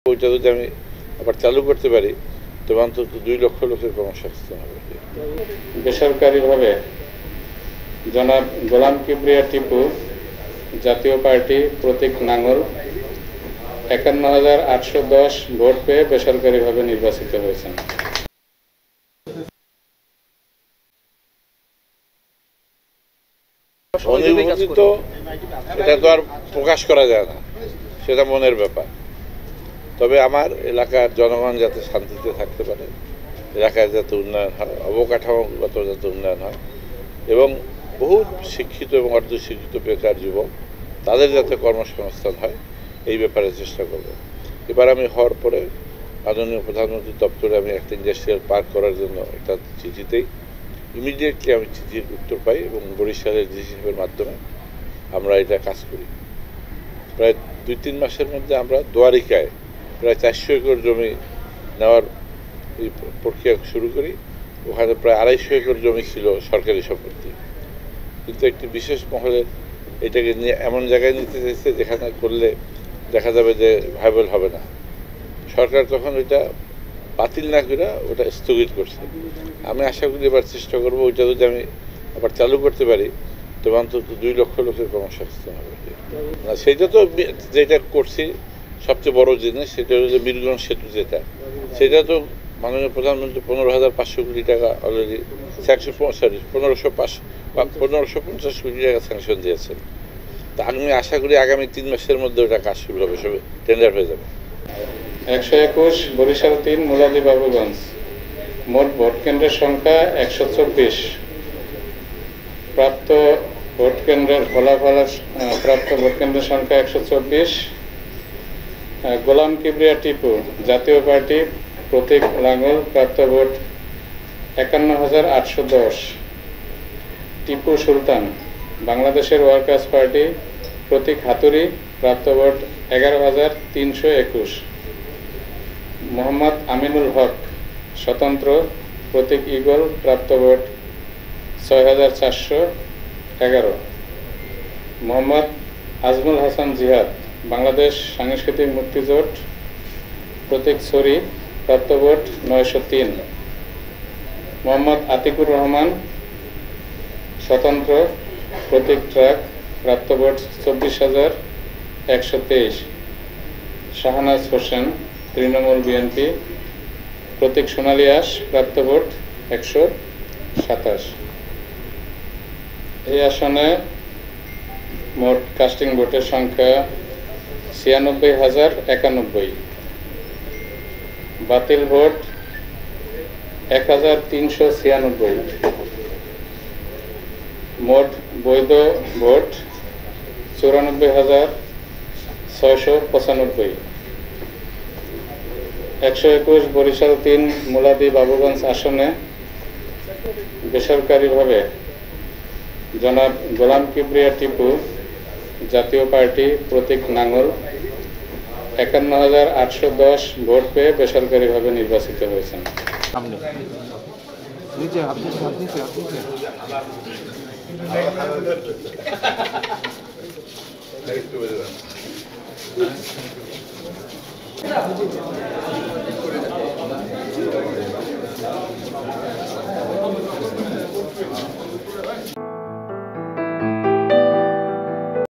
বরিশাল-৩ আসনে বিজয়ী হয়ে গোলাম কিবরিয়া টিপু যা বললেন | Election | News Barisal
বরিশাল-৩ (বাবুগঞ্জ-মুলাদী) আসনে লাঙ্গল প্রতীকের প্রার্থী গোলাম কিবরিয়া টিপুকে বিজয়ী ঘোষনা করার পর সাংবাদিকদের সাথে কথা বলেন।